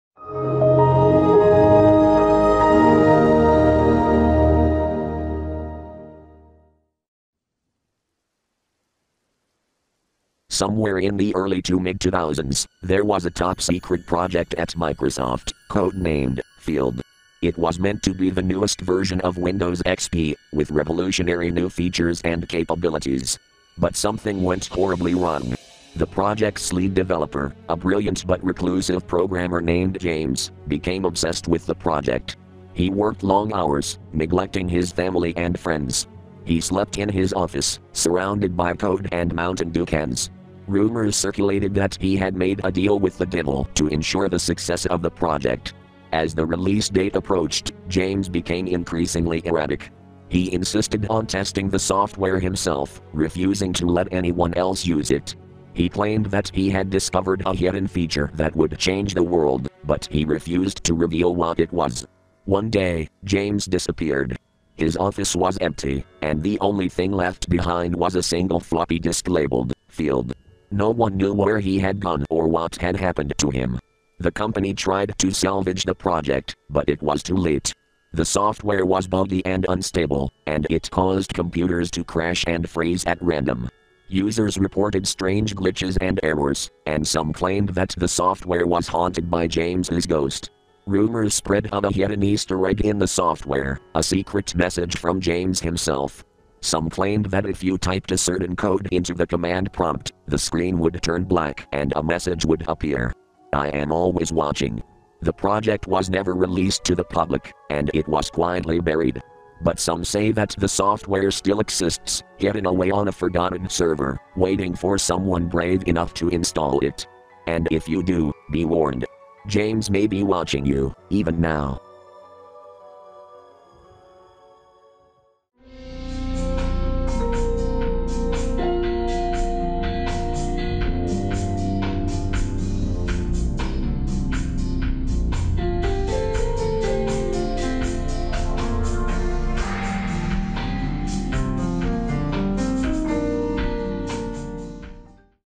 (Creepypasta) Windows XP Codename: Field (by KyleTheBestTheBest)
Yep, another Windows Creepypasta done entirely with an AI program, possibly You.com.

Credit: @Kyle123Cartoons_Age_Is_13_Fr and You.com

Sound FX from:
Chosic
Disney Wiki
FindSounds
FreeSFX
Freesound
SoundBible
Sounddogs
Soundeffects Wiki
Sound Ideas
Sound Image
Storyblocks
The Sounds Resource
Videvo